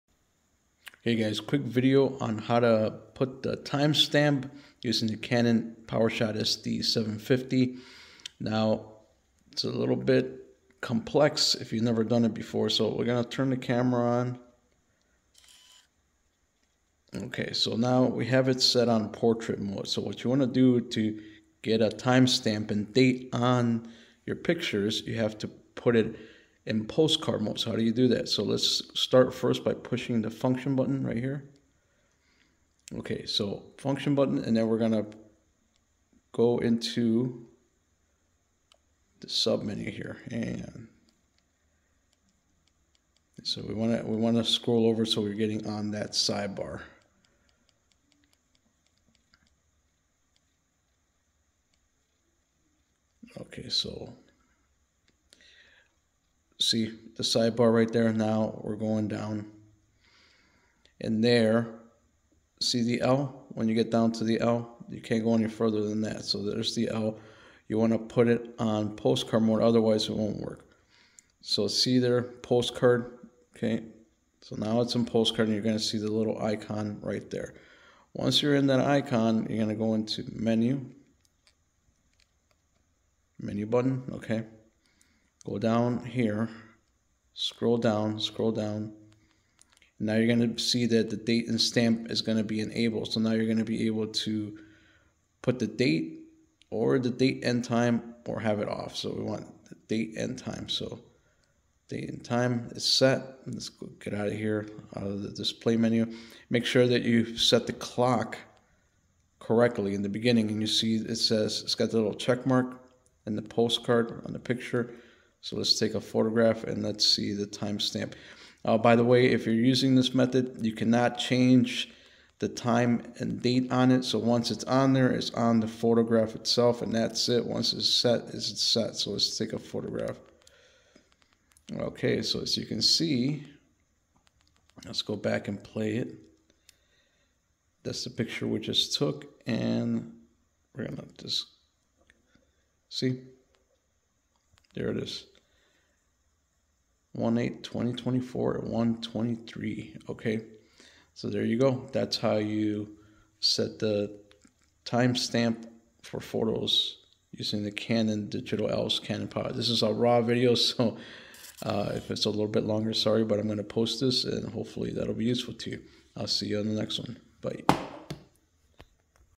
How to add Time and Date Timestamp Canon Powershot SD750 Tutorial Easy steps
In this video I will show you how to add a timestamp to your photos in the Canom Powershot SD750